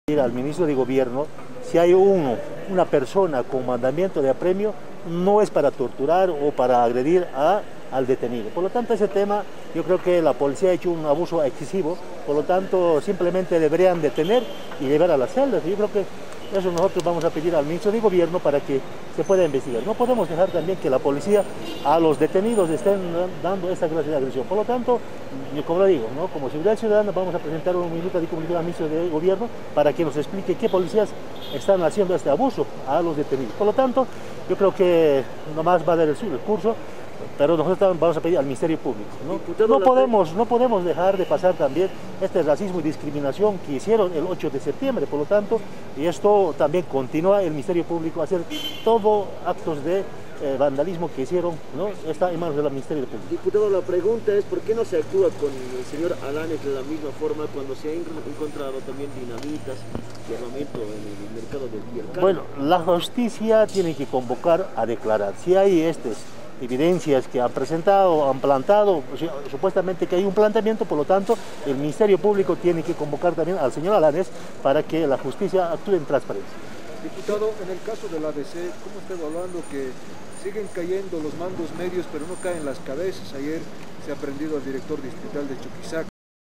23/09/2022 DIPUTADO ANDRÉS FLORES (MAS)
ASEVERA QUE LA POLICÍA HIZO UN ABUSO EXCESIVO EN LA DETENCIÓN DEL DIRIGENTE COCALERO CESAR APAZA POR ELLO SOLICITARÁN UN PETICIÓN DE INFORME AL MINISTRO DE GOBIERNO EDUARDO DEL CASTILLO, POR LOS ACTUADOS DE LA POLICÍA.